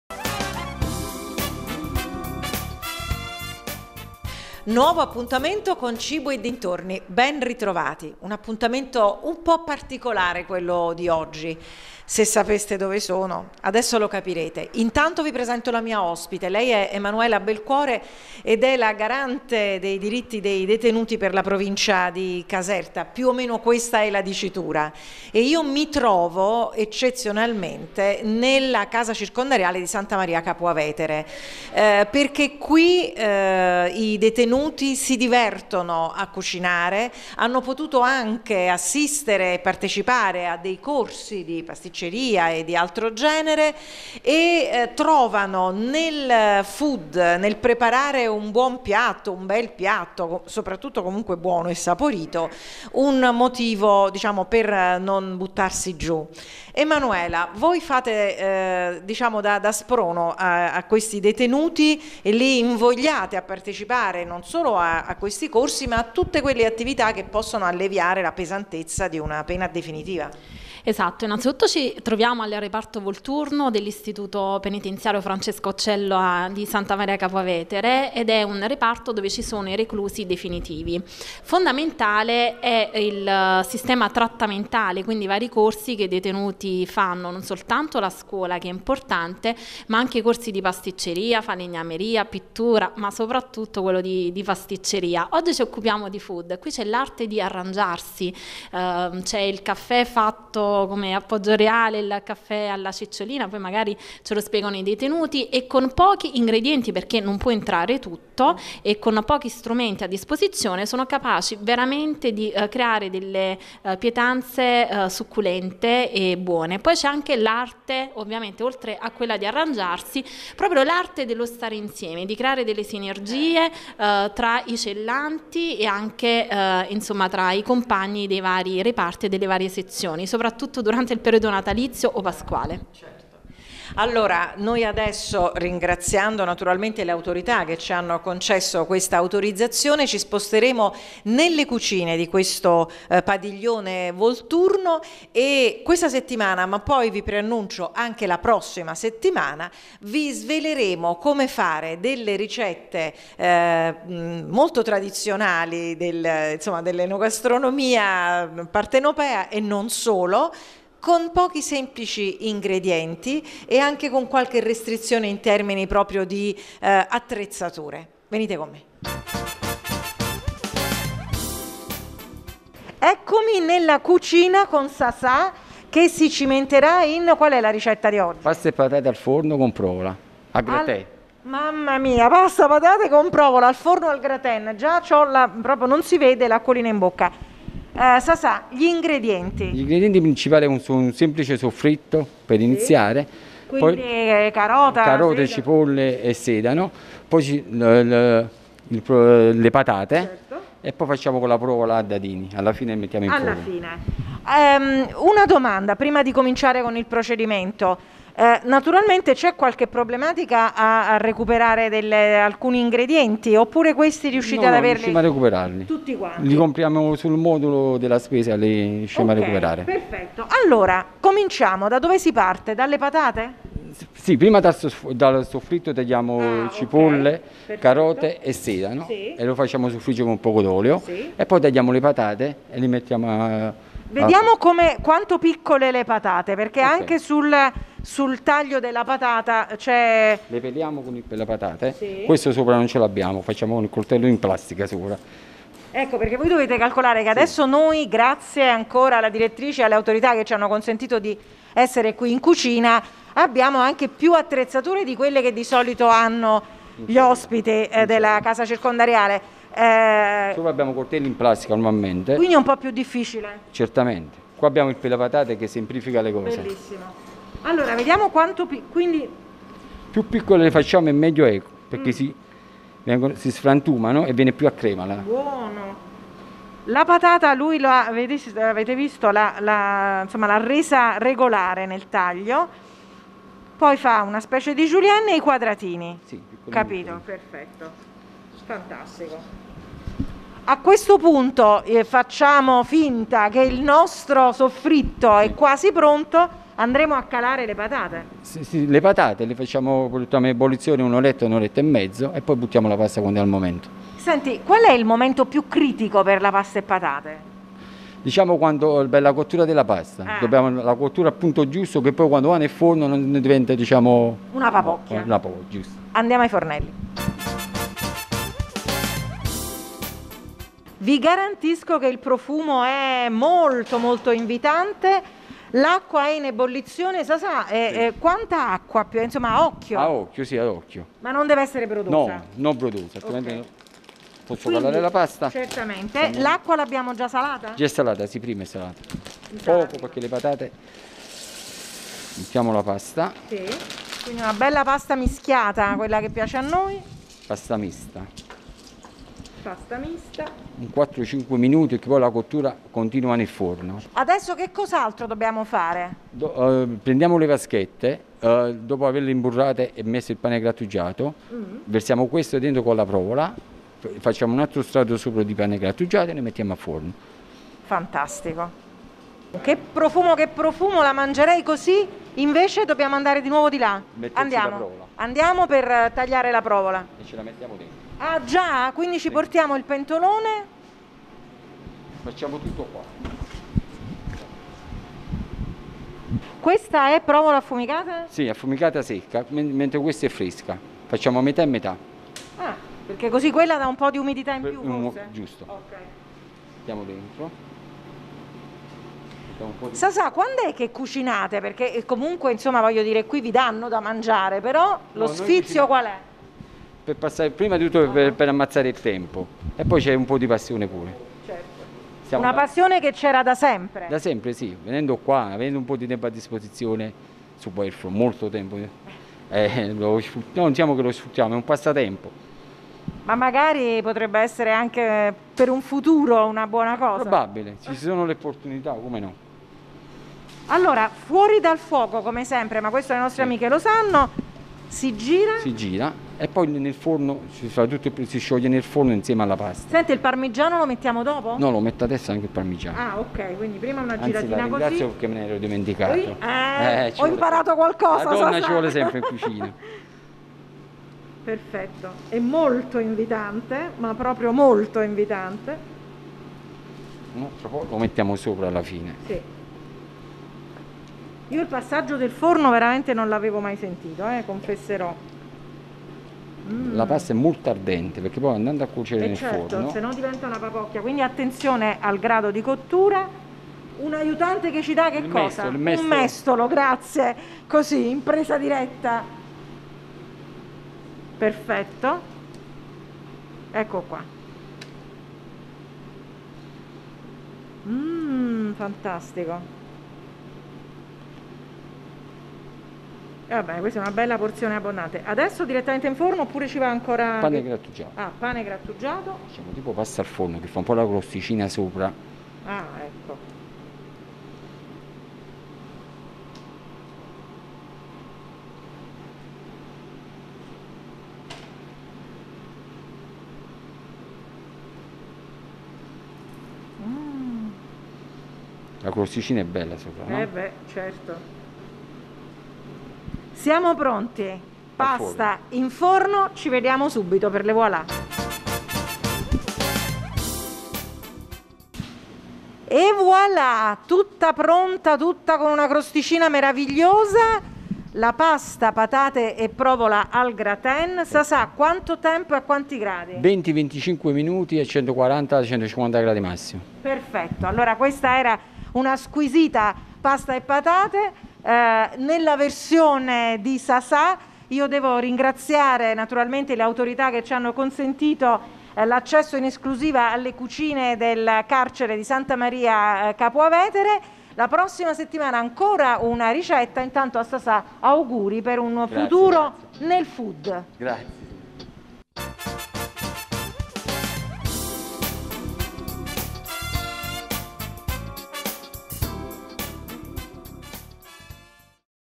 Pasta patate e provola
Ha il profumo della libertà questa pasta e patate  e provola resa croccante dalla doratura finale in forno. E ce la prepara uno chef un po' diverso dal solito...